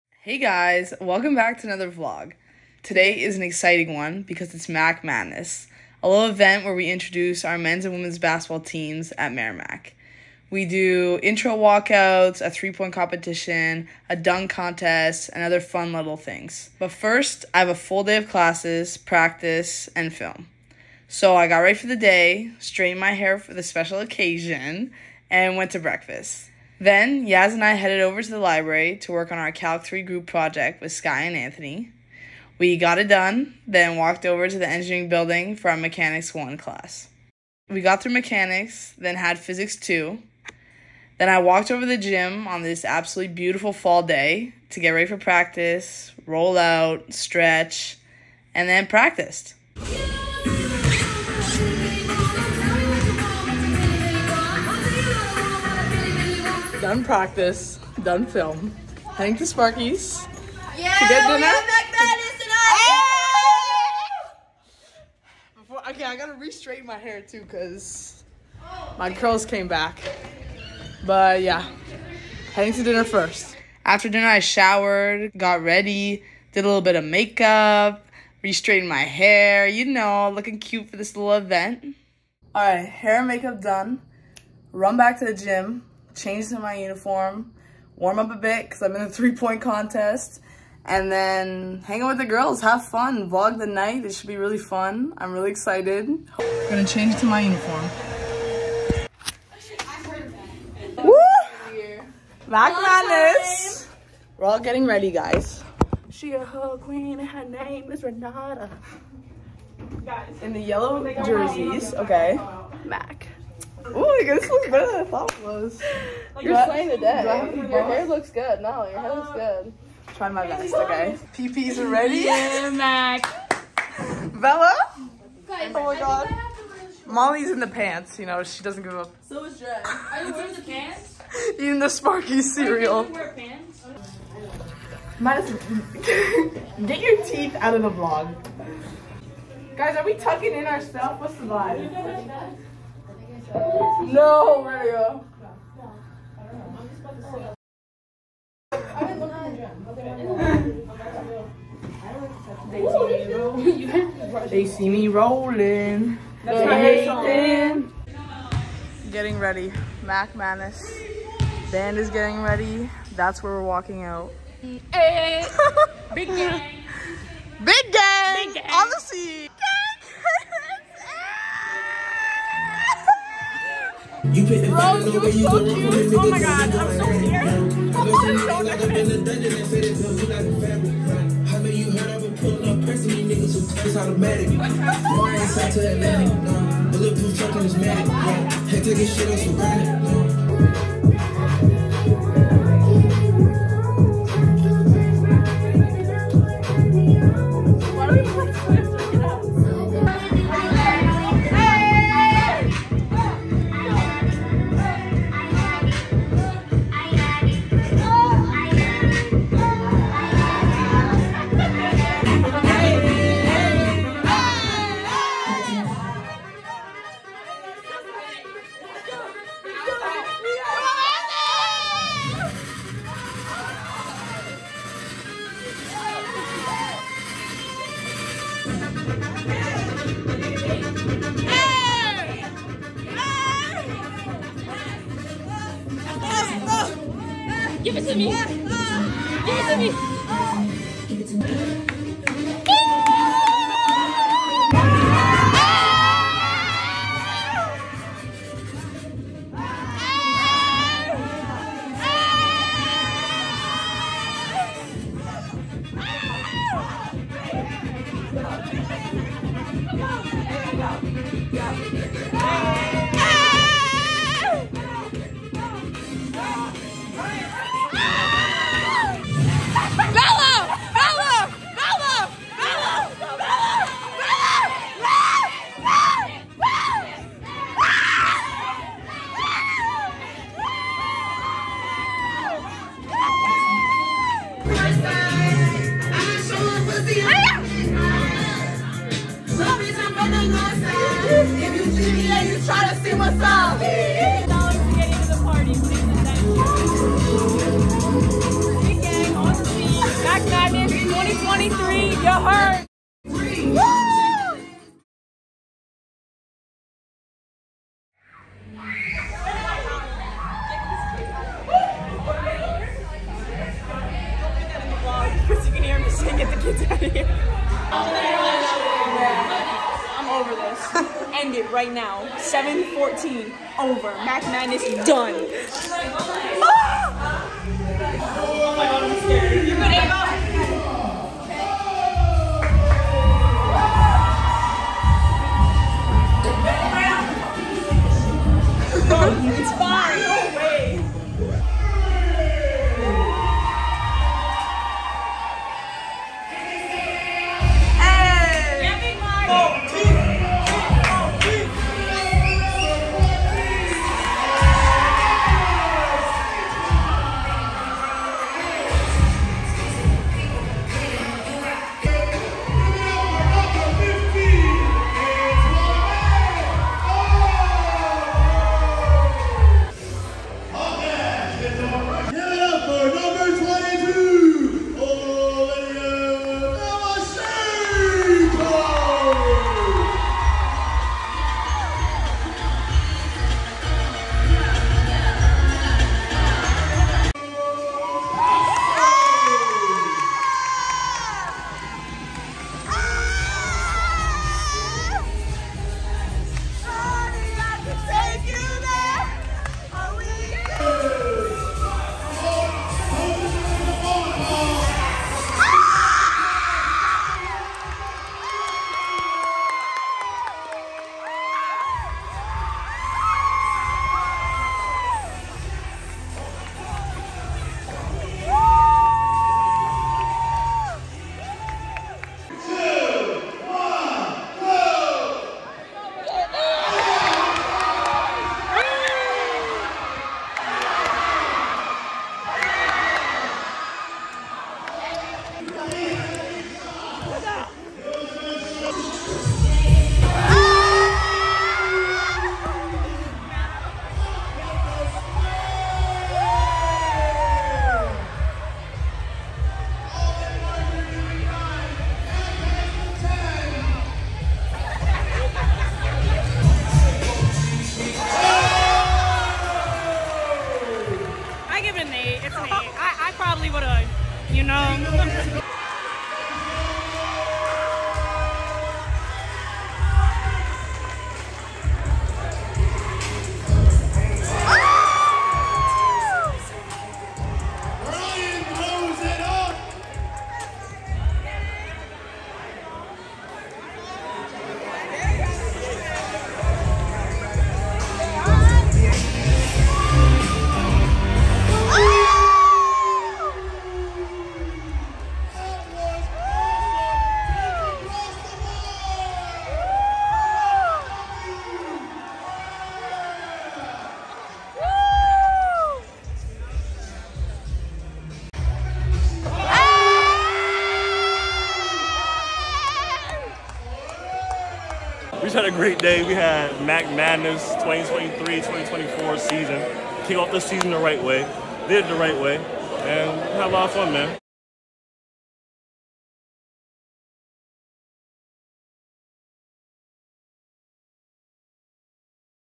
MACK MADNESS 2023! Intro Night for Merrimack Basketball
Hey guys!!! Welcome back to another vlog! Today I'm taking you behind the scenes of our Mack Madness event where we introduce the men's and women's basketball teams at Merrimack College!